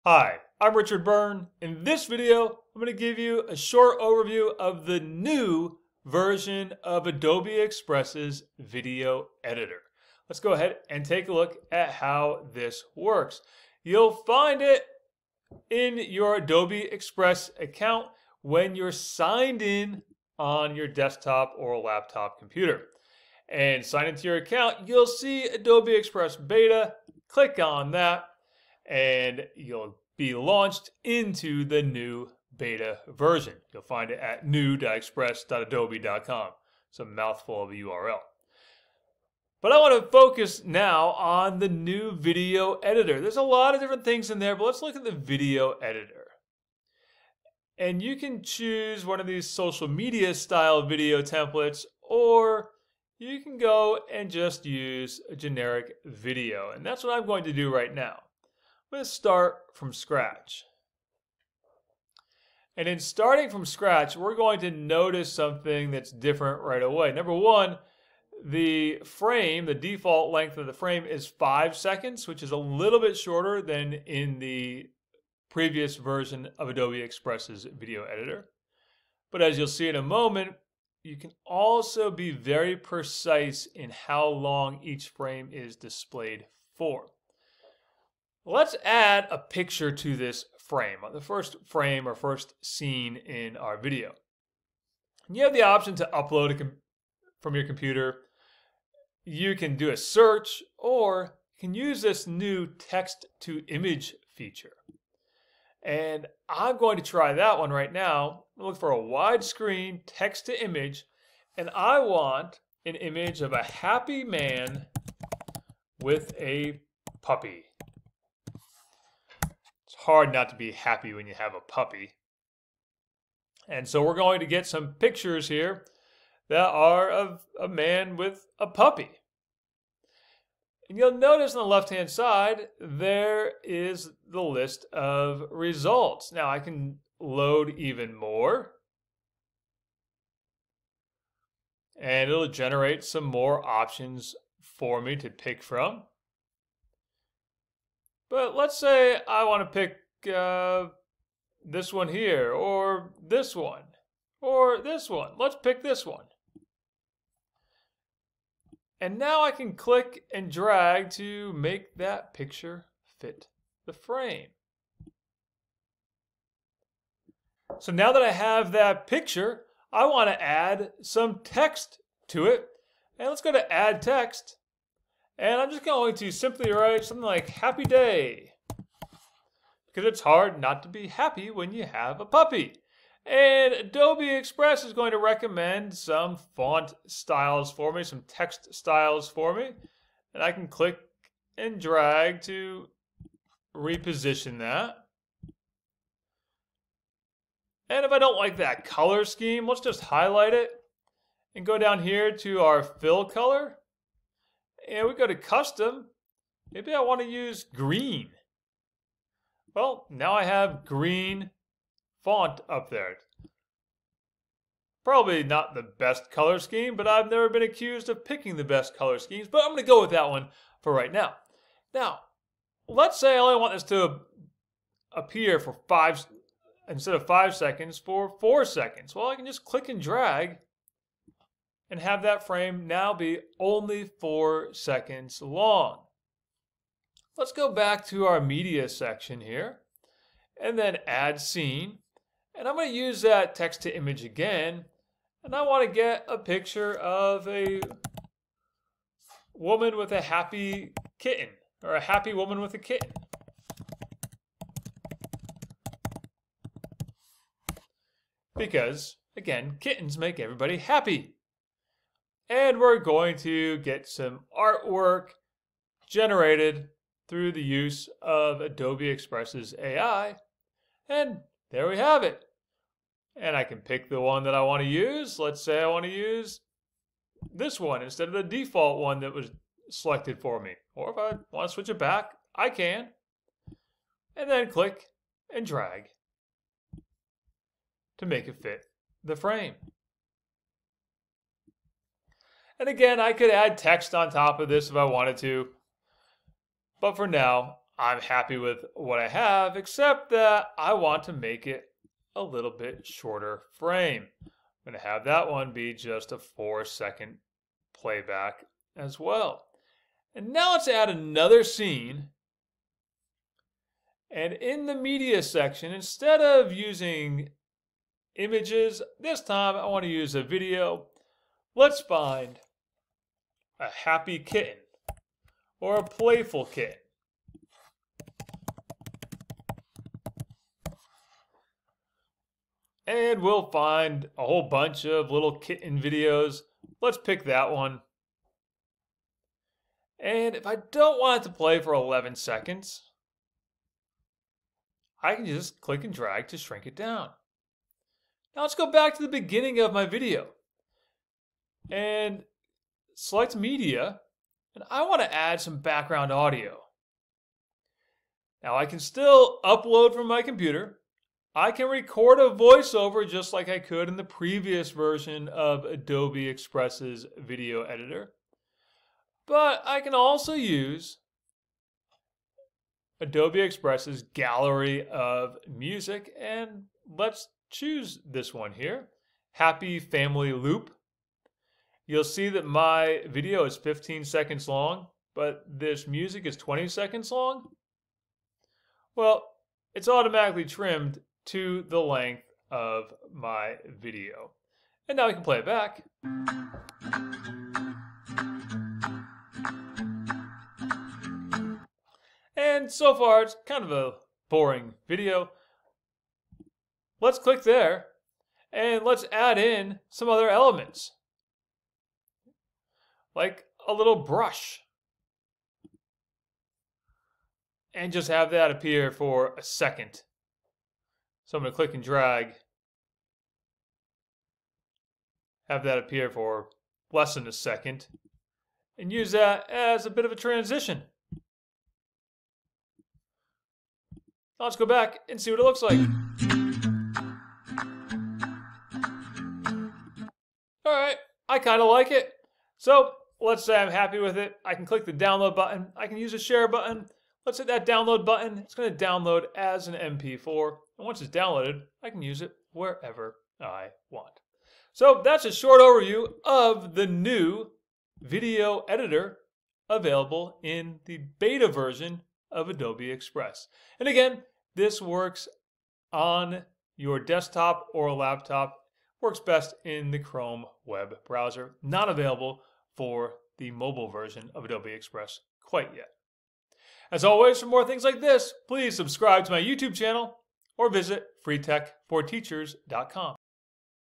How to Use the Adobe Express Video Editor
There's a new version of Adobe Express available! Highlights of the new Adobe Express video editor include improved control over frames, new text tools, better audio controls, and an AI-powered image generator!
Watch this video for an overview of how to use the new Adobe Express video editor.

📔 50 Tech Tuesday Tips!
🏫 Take a course with me: 🏫